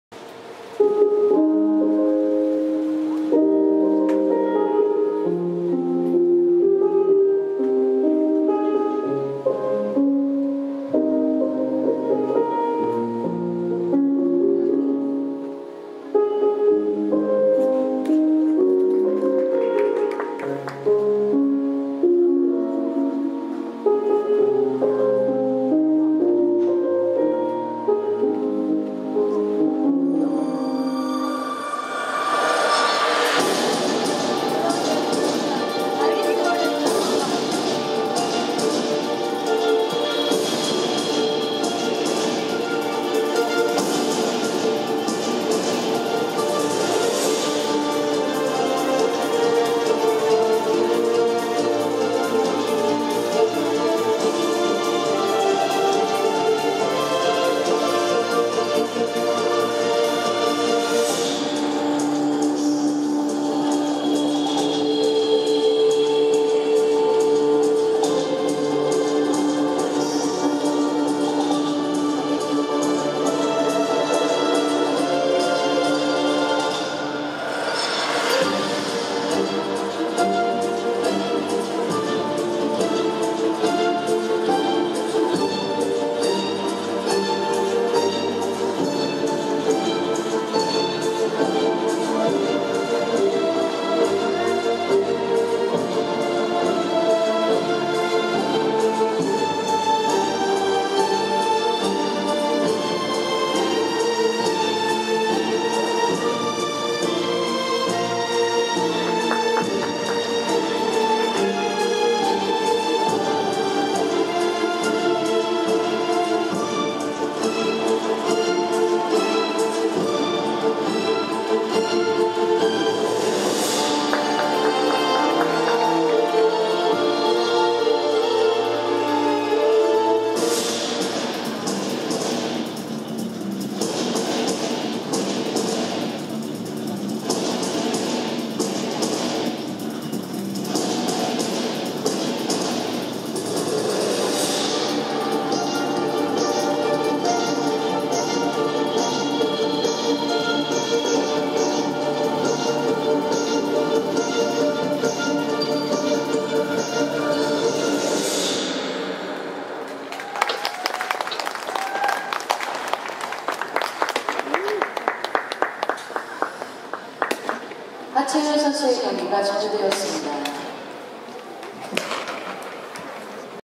여자1급 고학년부 FS 31 하채윤,서현초4 @ 2010년 피겨 꿈나무대회
2010/12/02, 2010년 제12회 전국남녀 피겨스케이팅 꿈나무대회 1일차, 태릉 실내빙상장